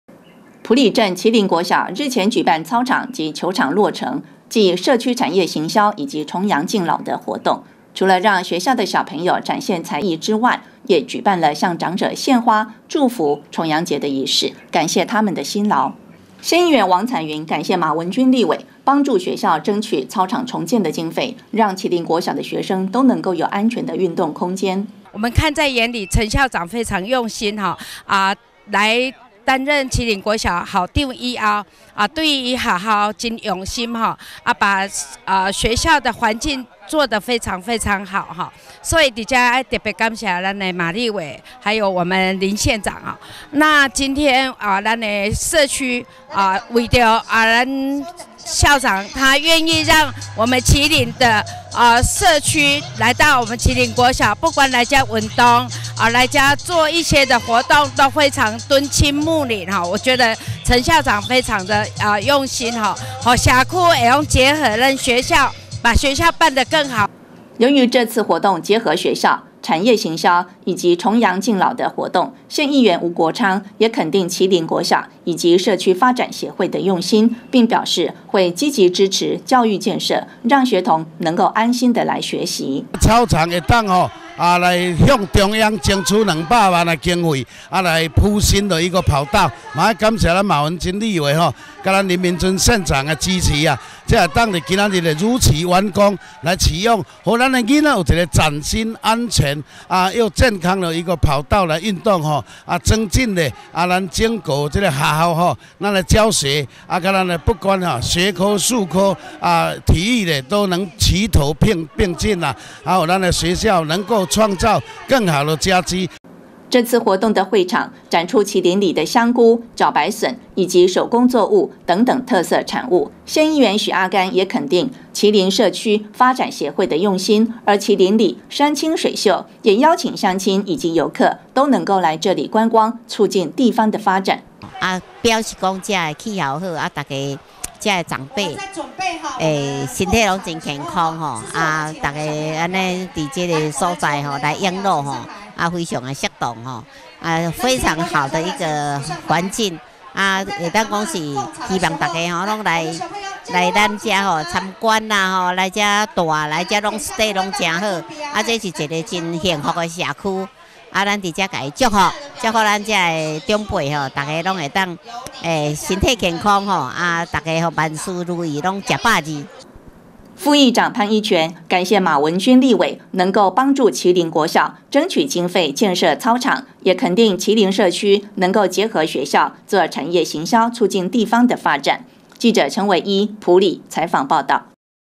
20171024民議新聞 麒麟國小操場落成啟用 結合產業行銷共同發展 (縣議員 王彩雲、吳國昌、許阿甘)
埔里鎮麒麟國小日前舉辦操場啟用暨社區產業行銷以及重陽敬老的活動，縣議員許阿甘、議員王彩雲、議員廖志城、議員吳國昌也都到場參加這場盛會。副議長潘一全感謝馬文君立委幫助爭取經費，讓學生都能夠擁有安全的運動空間，也肯定麒麟社區能夠結合產業行銷活動，促進地方的發展